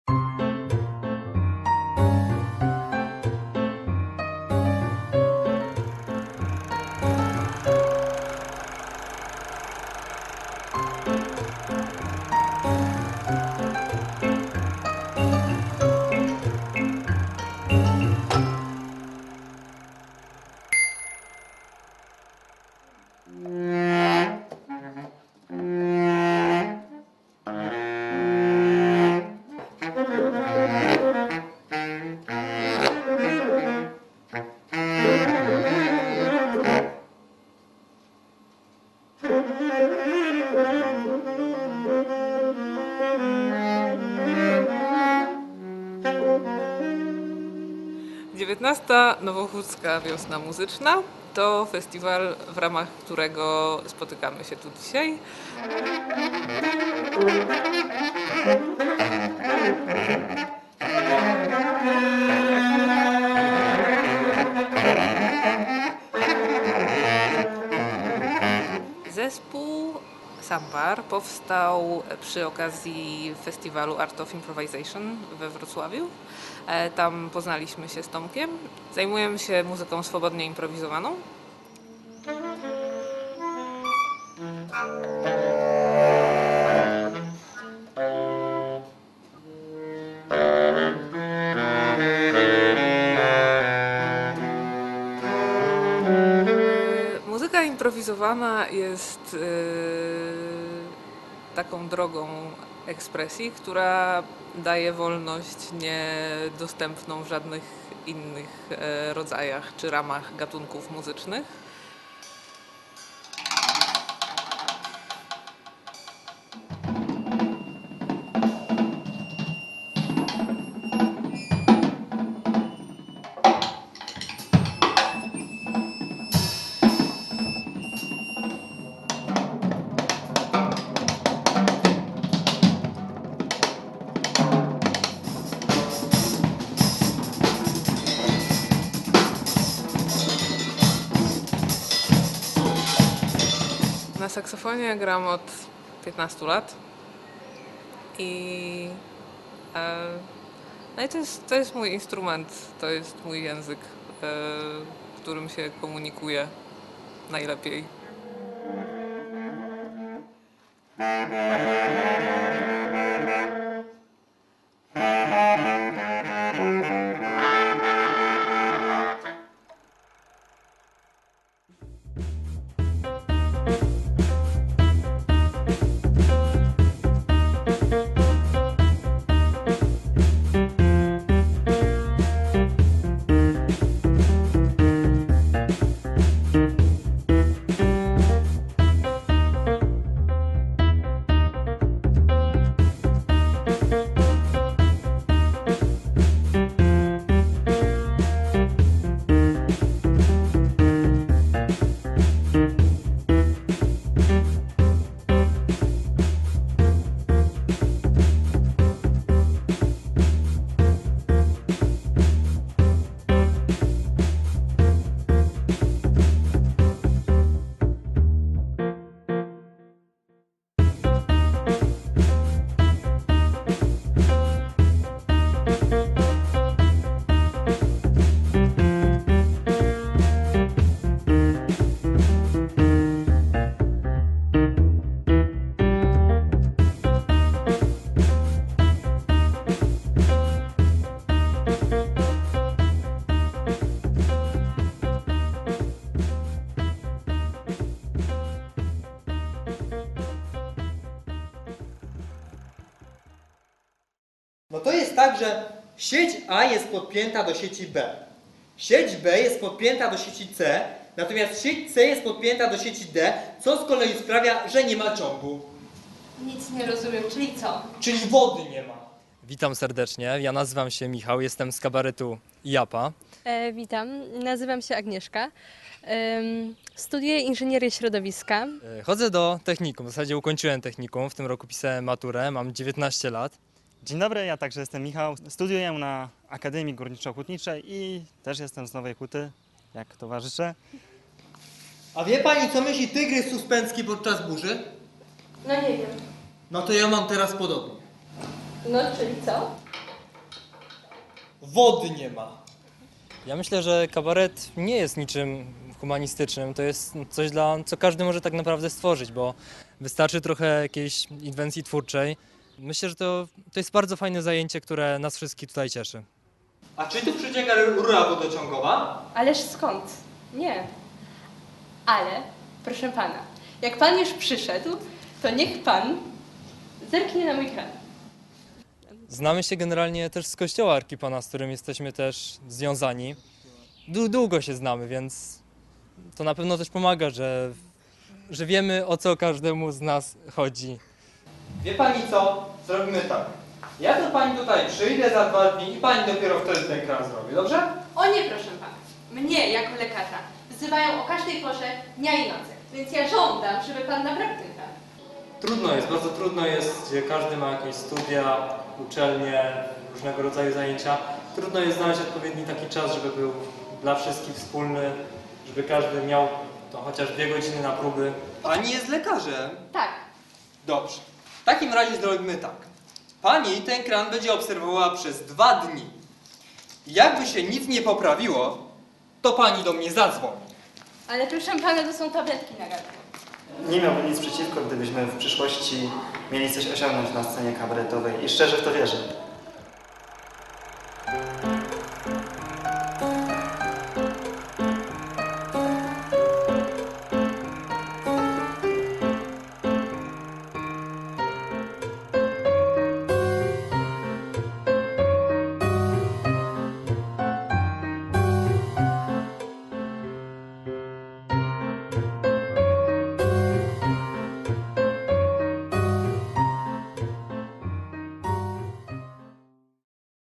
Nowohucka Kronika Filmowa nr 384
MUZYCZNA WIOSNA
W ramach XIX Nowohuckiej Wiosny Muzycznej w Klubie Kuźnia wystąpił duet SAMBAR z koncertem muzyki swobodnie improwizowanej. Grupę tworzą Paulina Owczarek i Tomek Gadecki. Paulina opowiada o zespole i wolności, jaką daje tego rodzaju muzyka.

SKATEPARK
Skatepark w Mistrzejowicach to jeden z największych i najlepiej opracowanych krakowskich obiektów tego typu. Powstał w odpowiedzi na zwiększającą się liczbę osób uprawiających w Nowej Hucie sporty extremalne. Stanowi miejsce ćwiczeń i udanych (lub nie!) prób młodych ludzi próbujących ewolucji na deskorolkach czy rowerach.

JAPA
Portret początkującego kabaretu JAPA, który tworzą Michał, Agnieszka i Michał – absolwenci nowohuckich szkół średnich. Ich marzeniem jest stworzenie zespołu, który coś osiągnie na scenie kabaretowej.

🎥 Realizacja:  Grzegorz Zariczny, Klaudyna Schubert, Magda Urbańska